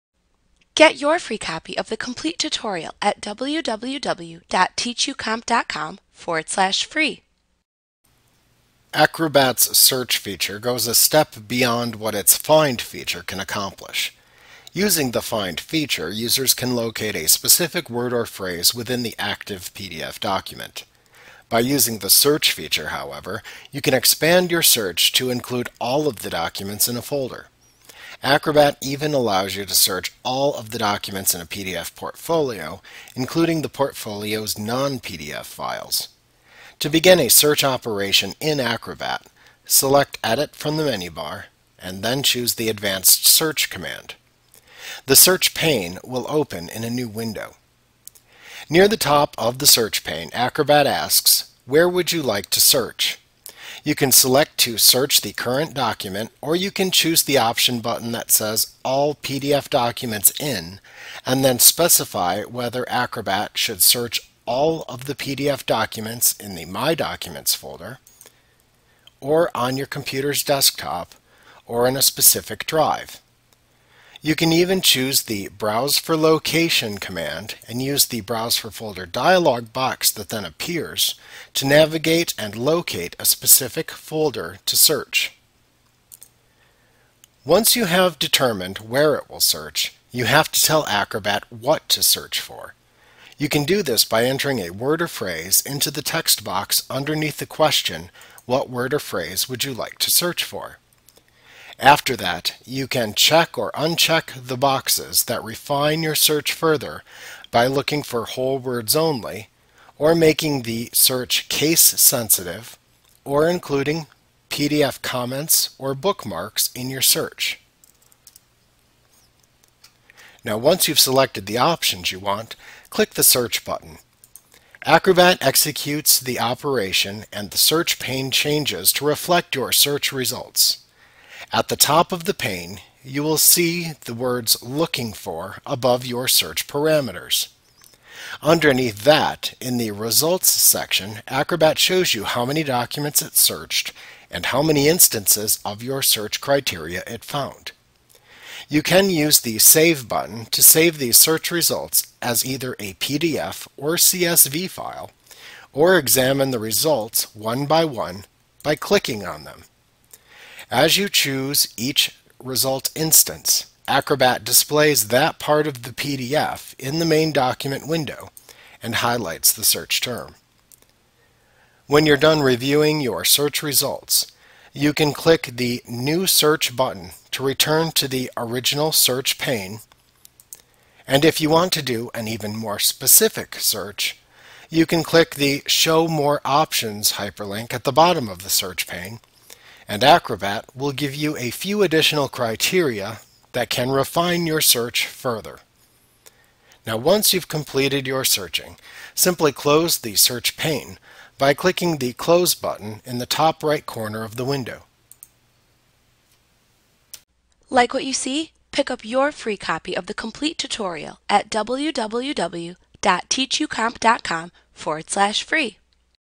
Searching a PDF and Using the Search Panel - Adobe Acrobat XI Training Tutorial Course
Learn about Searching a PDF and Using the Search Panel in Adobe Acrobat XI Visit us today!